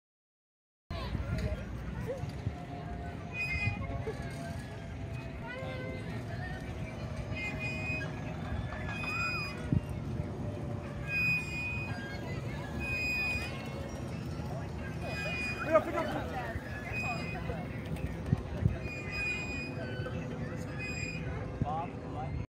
Ellie swinging at Santa Monica Beach
Never too old to Swing, especially at Santa Monica Beach Aug 2017